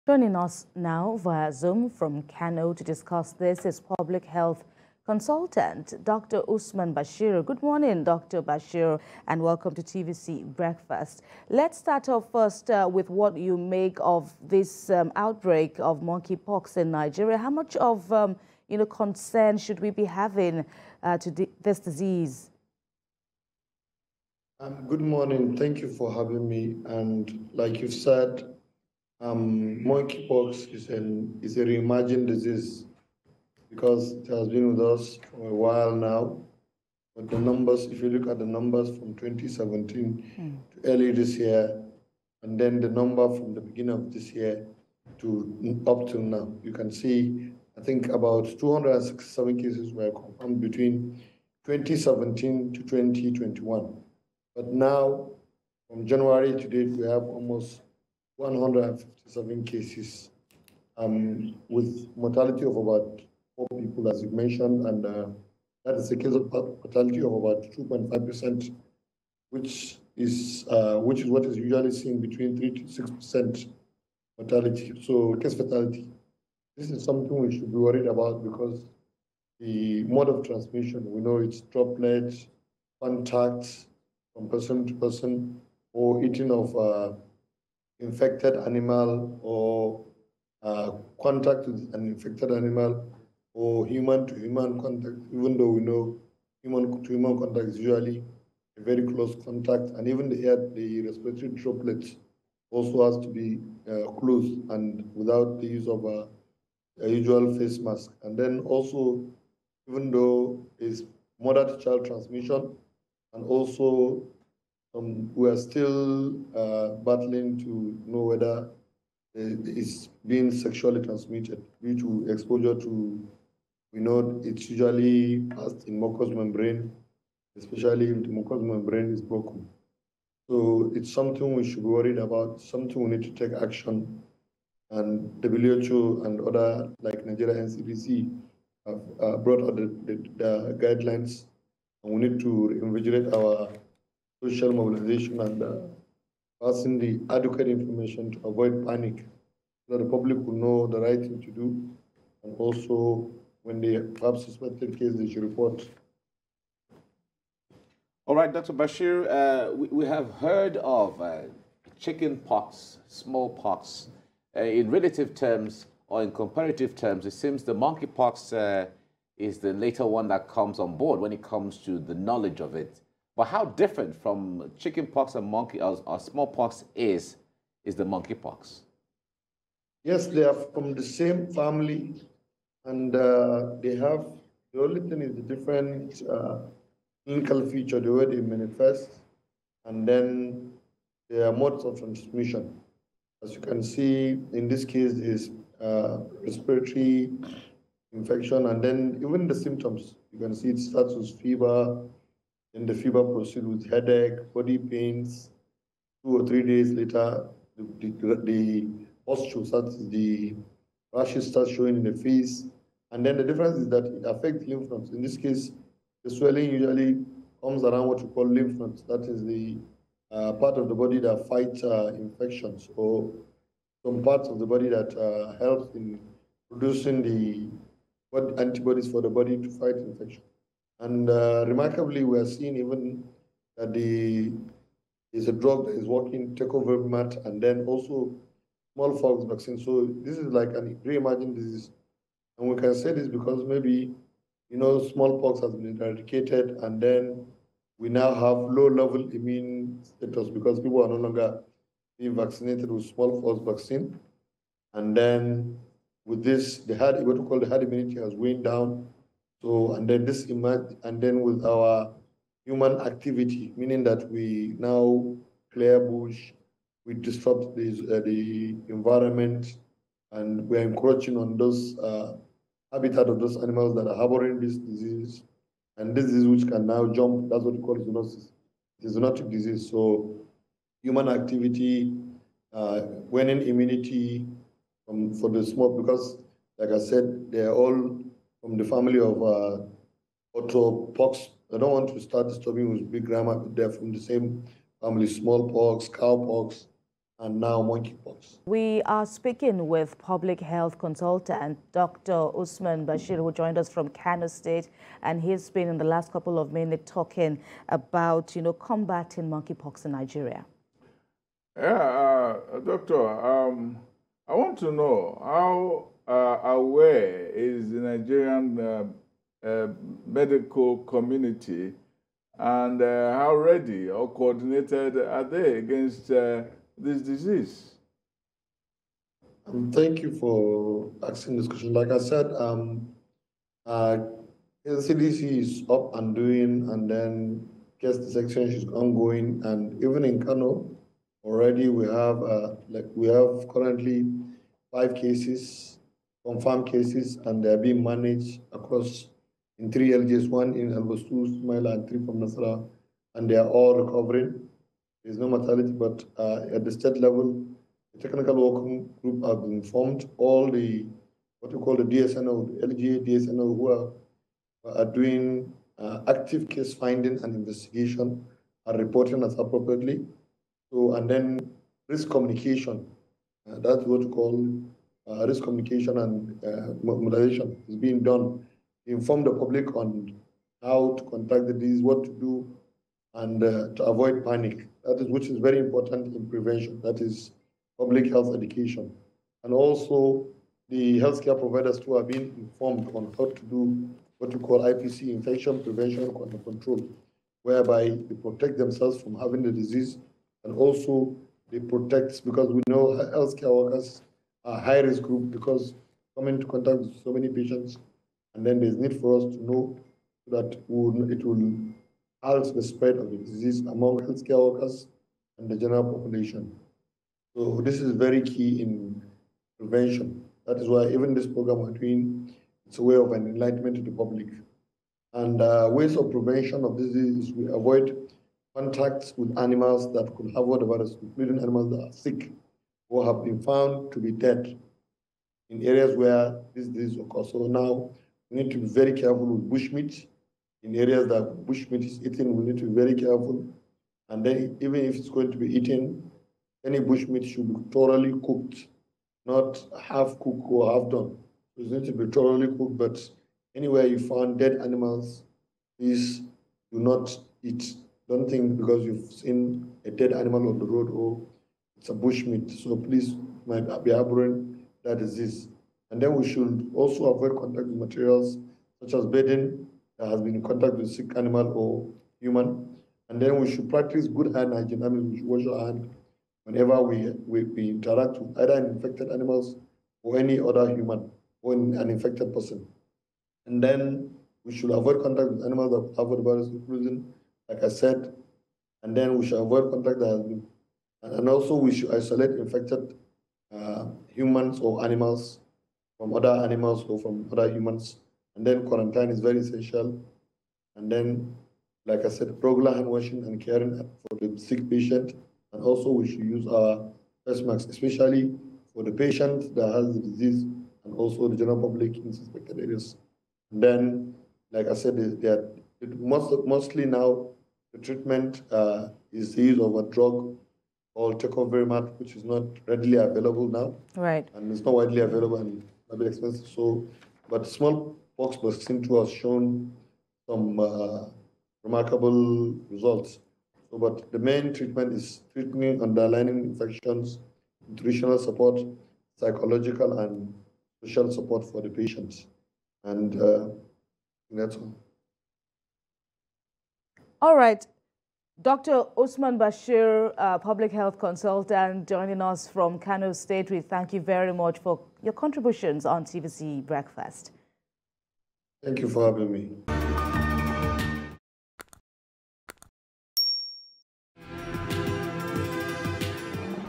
Dr. Usman Bashir Analyses NCDC Confirms 157 Monkeypox Cases
This is according to the Nigeria Centre for Disease Control (NCDC).
The NCDC said the cases were recorded across 26 states since January with four deaths reported from January 1 to July 31, in 4 states – Delta (1), Lagos (1), Ondo (1) and Akwa Ibom (1).

Public health consultant, Dr. Usman Bashir discuss Monkeypox Cases, solution and more in Nigeria